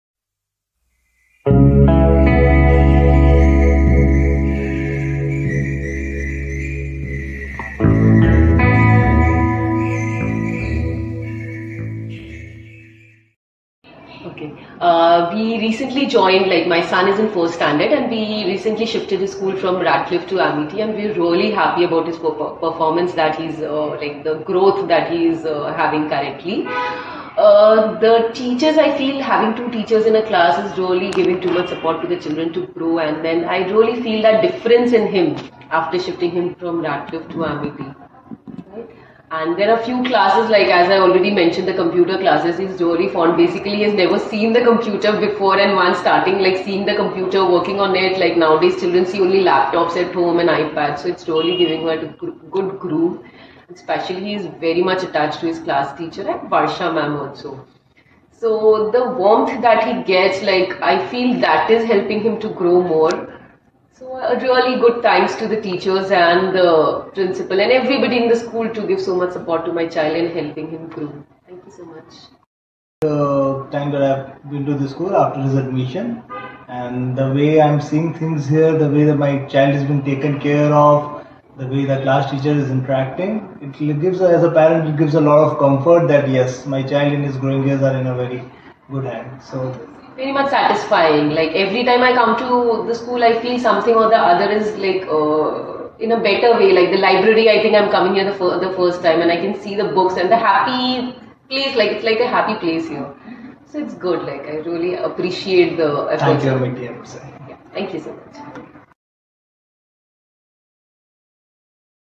Parent Testimonial
Hear what our existing parents have to say about their experience in AISNM